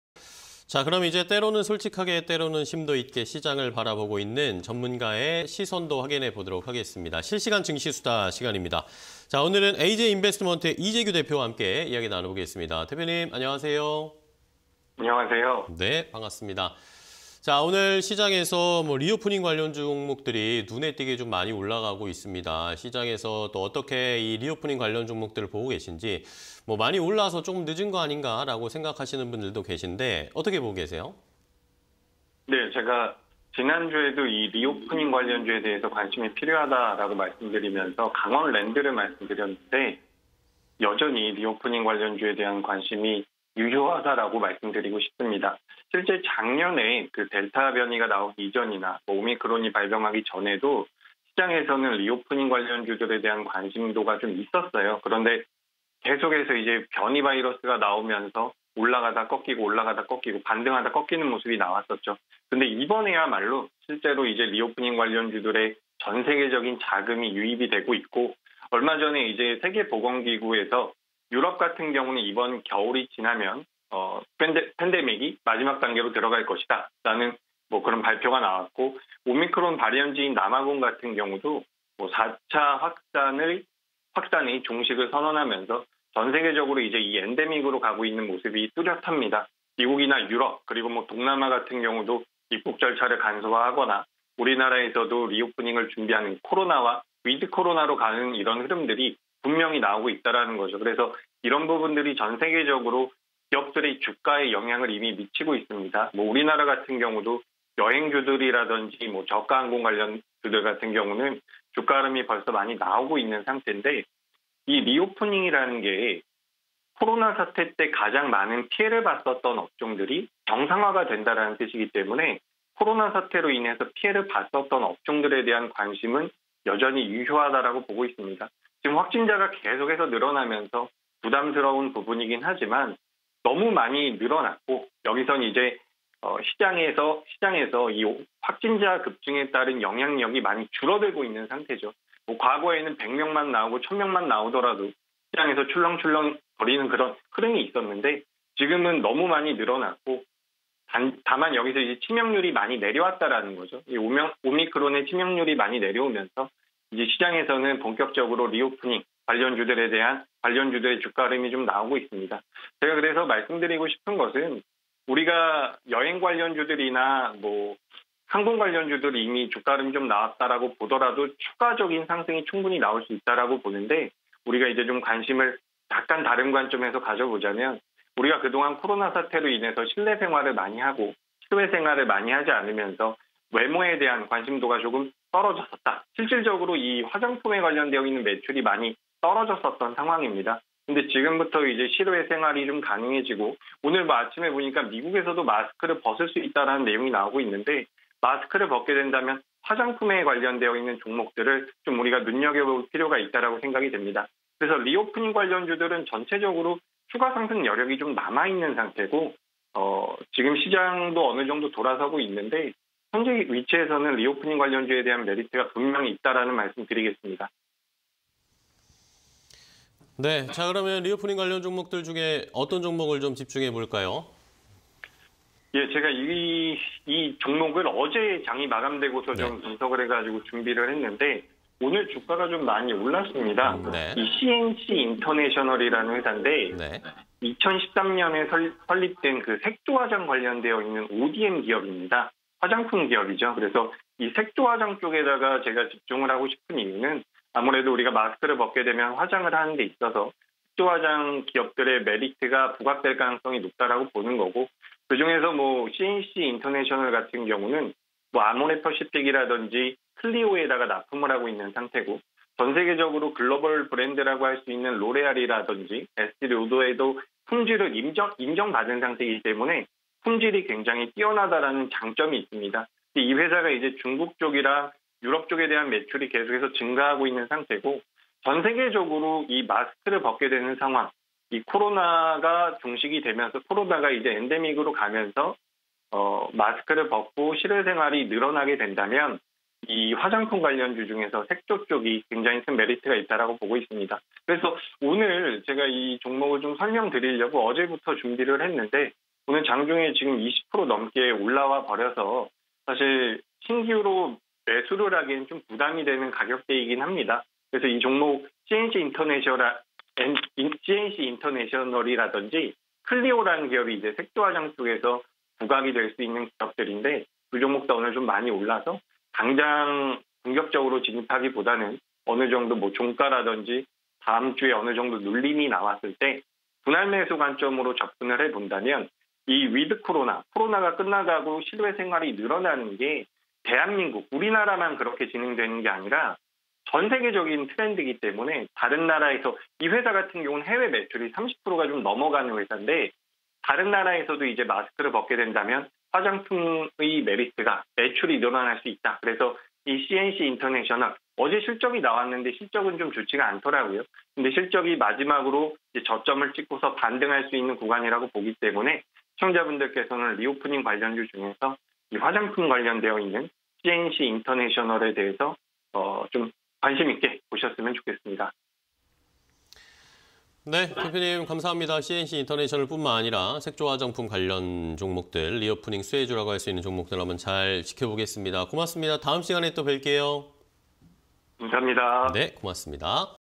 씨앤씨인터내셔널(352480)... 코로나 종식 다가온다… 주목할 리오프닝주는?_실시간 증시 수다 (20220217)
Q. 코로나 종식 다가온다… 주목할 리오프닝주는?
- WHO "겨울 이후 유럽은 팬데믹 마지막 단계"
- '오미크론 발현지' 남아공, 4차 확산 종식 선언
- 美·유럽 등 방역수칙 해제… 엔데믹 선언
- 동남아, 입국 간소화 등 리오프닝 움직임
- 국내, 방역 완화 등 위드 코로나 가능성
- 화장품, 실외 생활·No 마스크 시 수요 증가 전망

씨앤씨인터내셔널(352480)
- 색조 전문 ODM 업체
- 리오프닝 시 수혜 가능성
- 아모레퍼시픽·클리오 등 납품
- 위드 코로나 시대 실적 개선 기대
- 고마진 제품 위한 인력 대거 채용